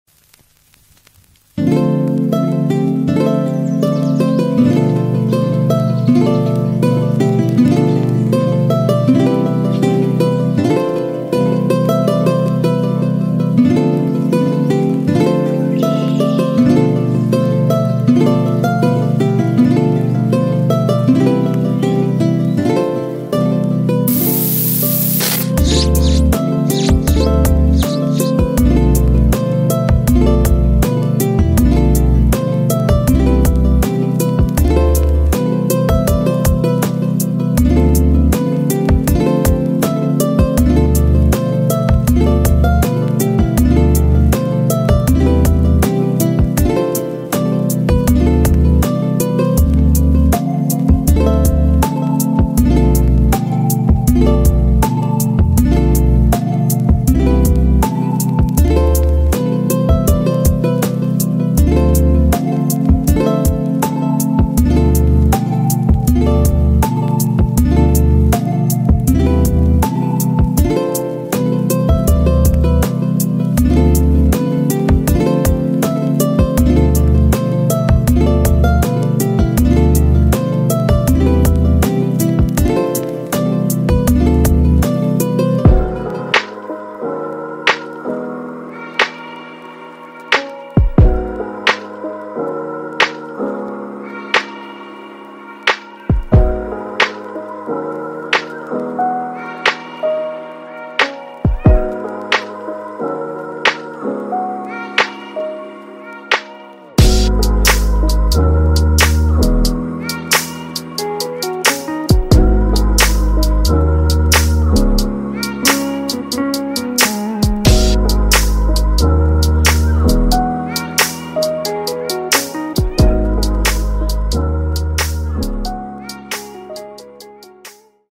Panço ❤️ Evimizin yeni neşesi
Evimizin yeni üyesi panço❤️ Araba motorundan evimize uzanan güzel bir yolculuk..